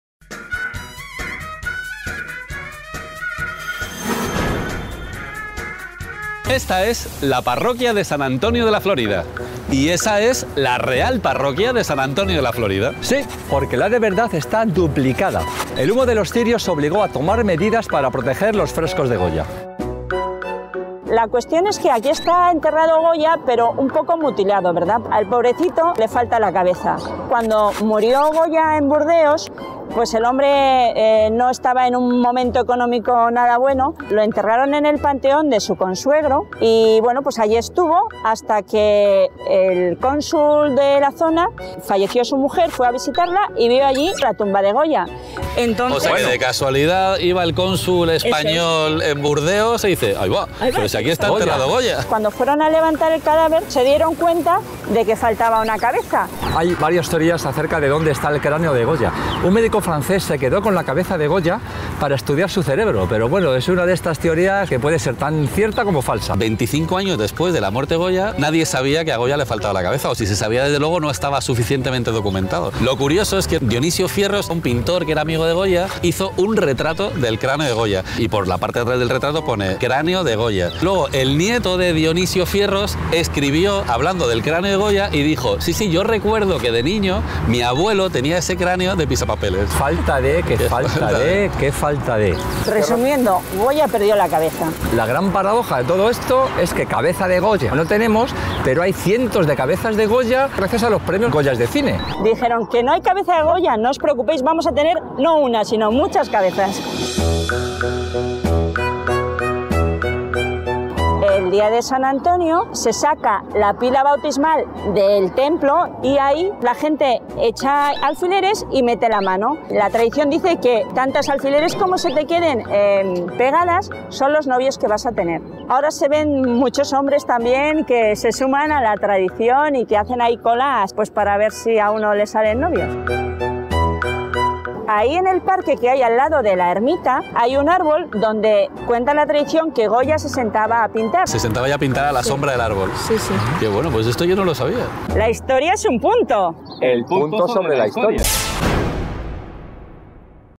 En San Antonio de la Florida Goya va de cráneo
Cerca de la ribera del Manzanares se levanta la parroquia de San Antonio de la Florida. En su interior se encuentran las impresionantes pinturas al fresco que Goya realizó en el interior de la cúpula. Don Francisco reposa junto a sus pinturas.
Seguramente estaría feliz si lo supiera. Pero... ah, cosas de España, a nuestro Goya le falta la cabeza.

La historia de tan respetable cráneo es misteriosa y a la vez cómica. Cuando España pidió a Francia la devolución de los restos, los gabachos no enviaron la cabeza. Simplemente no sabían dónde estaba. Las teorías son muy entretenidas. Según una de ellas, el cráneo acabó en manos de un médico francés que quiso estudiar las medidas de la genial testa. En aquella época estaba muy de moda la costumbre de medir cráneos e intentar deducir la personalidad del finado.

Otra bonita teoría. Parece ser que el amigo de Goya, el también pintor Dionisio Fierros, tenía el cráneo de Goya en su casa. ¿Cómo creéis que usaba el cráneo? ¿Decorando la chimenea? ¿Lo usaban en casa para ensayar textos de Shakespeare? No. ¡Lo usaba de pisapapeles! Triste destino el de un genio cuyo cráneo sirve para que no se vuelen unos papeluchos.